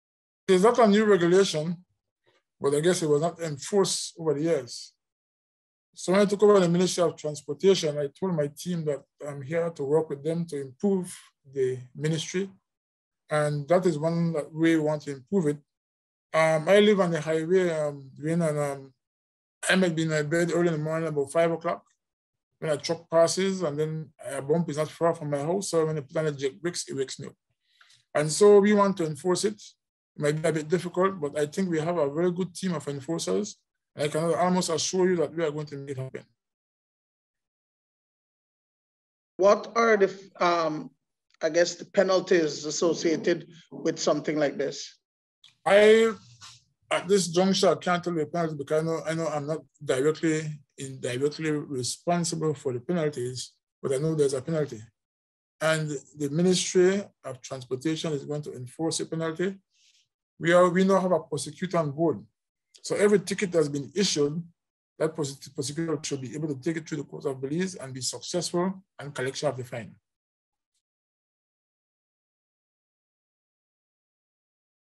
Caution: Excessive Noise from Motor Vehicles
If you are proud of the loud noise made by your truck, bus, car, motorcycle or moving equipment, or have even had work done to make it even louder in order to make your presence known, the following message is directed at you. Today the Department of Transport through the Ministry of Public Utilities, Energy and Logistics is warning owners and drivers of tractor trucks and motorcyclists about noise pollution. The Department says any vehicle that causes excessive noise is prohibited by law, under the Motor Vehicles and Road Traffic Regulations. Today, Minister Rodwell Ferguson told the media that it is a public nuisance that affects community residents and he, himself, has experienced it.